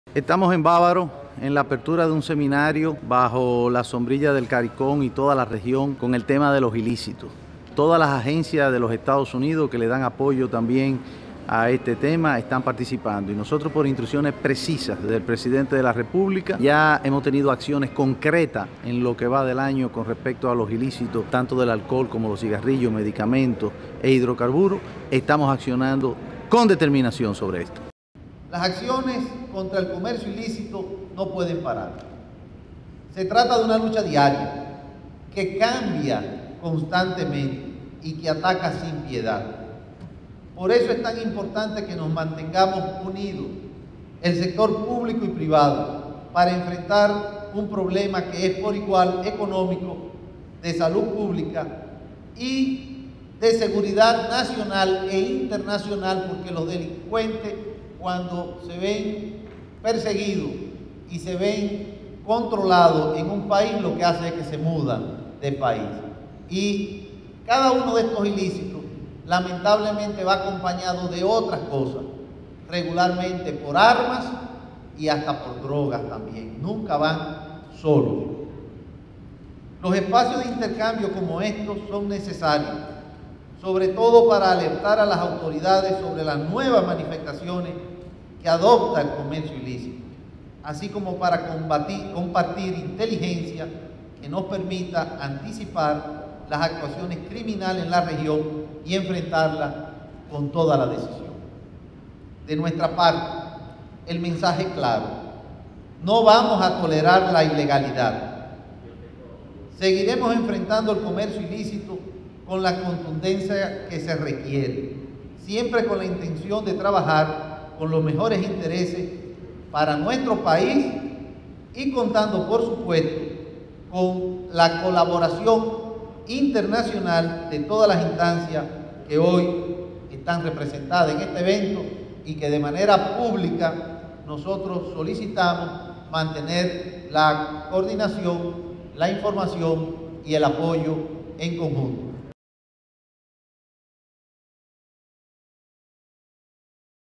¡No vamos a bajar la guardia con la ilegalidad!
02 de Agosto de 2021

En coordinación con el Departamento de Seguridad Nacional de los Estados Unidos (@dhsgov) quedó inaugurado el entrenamiento sobre Aplicación de Derechos de Propiedad Intelectual y Comercio Global‘’ que tendrá una duración de 5 días y quienes participan podrán conocer las mejores prácticas relacionadas con el combate al comercio ilícito.

Desde el principio nos hemos enfocado en combatir esta problemática por lo que se han definido canales directos de comunicación e intercambio de información con @aduanasrd, la @procuraduriagralrd, @saludpublicard, @mdefensard , @dgii.rd y los diversos organismos de investigación del Estado, para articular respuestas ágiles y oportunas por parte de los organismos públicos.